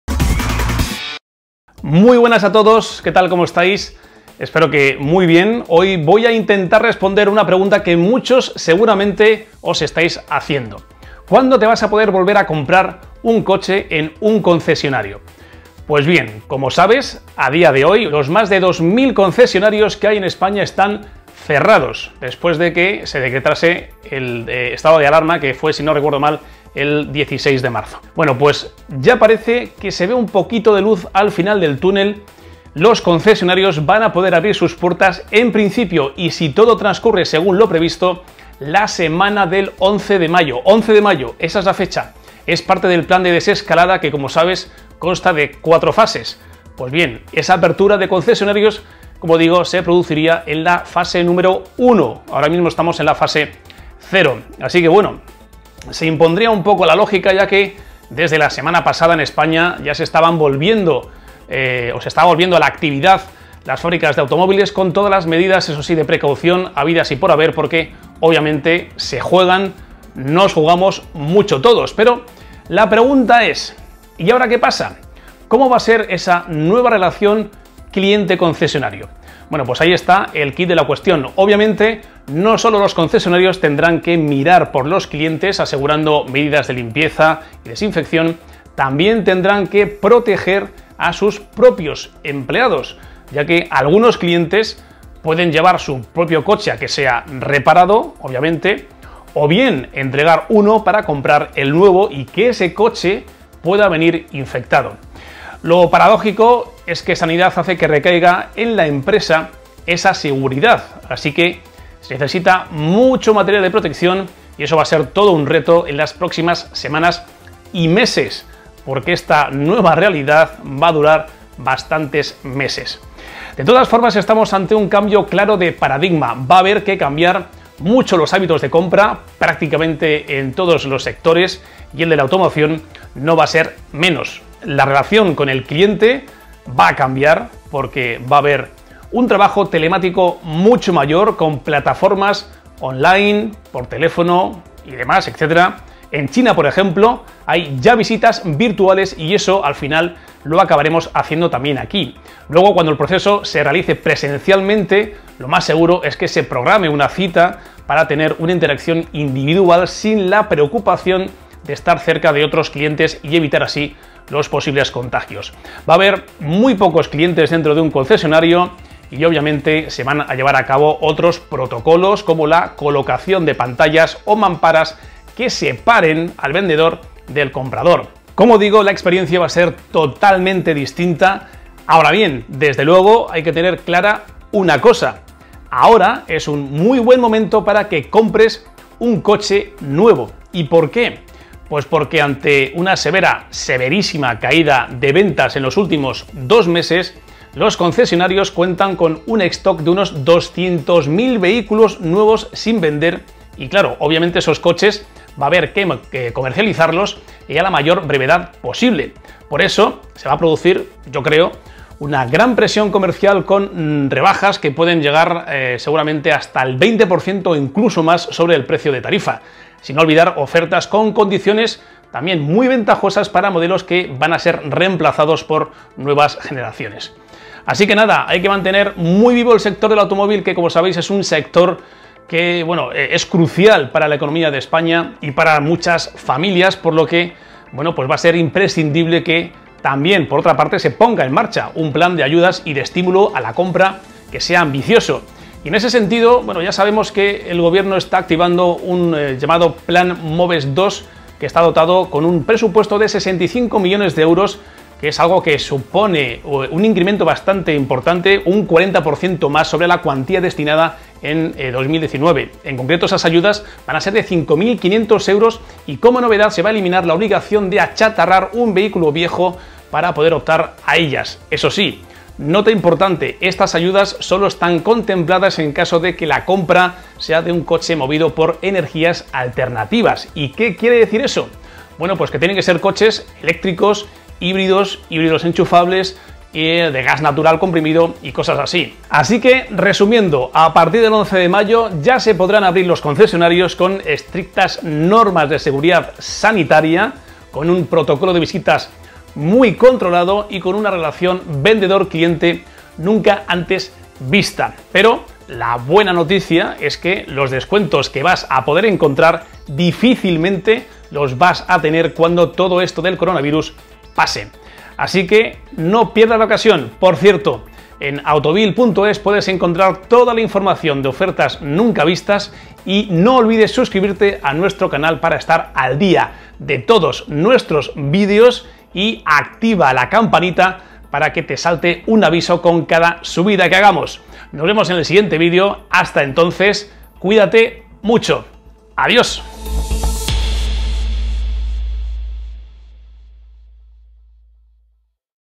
¿CÓMO va a ser COMPRAR UN COCHE tras el CONFINAMIENTO?
A partir de la semana del 11 de mayo, y si todo transcurre según lo previsto, los concesionarios volverán a abrir sus puertas, aunque ya nada volverá a ser como antes, al menso durante unos cuantos meses.
La relación con el cliente va a cambiar. Porque va a haber un trabajo telemático mucho mayor, con plataformas online. En China, por ejemplo, ya hay visitas virtuales para la compra de un coche y eso lo acabaremos haciendo aquí también cuando se desarrolle.
Si el proceso se realiza presencialmente, lo más seguro es que se programe una cita para tener una interacción individual sin la preocupación de estar cerca de otros clientes y evitar posibles contagios. Va a haber muy pocos clientes dentro de un concesionario. Y obviamente, se van a llevar a cabo otros protocolos como la colocación de pantallas o mamparas que separen al vendedor del comprador.
Ahora bien, desde luego, hay que tener clara una cosa: ahora es un muy buen momento para que compres un coche nuevo. ¿Por qué? Porque ante una severa caída de ventas en los últimos dos meses los concesionarios cuentan con un stock de unos 200.000 vehículos nuevos sin vender, y esos coches habrá que comercializarlos a la mayor brevedad posible. Por eso, se va a producir una gran presión comercial con rebajas que pueden llegar seguramente al 20% o incluso más del precio de tarifa. Sin olvidar ofertas con condiciones muy ventajosas para modelos que vayan a ser reemplazados por la nueva generación.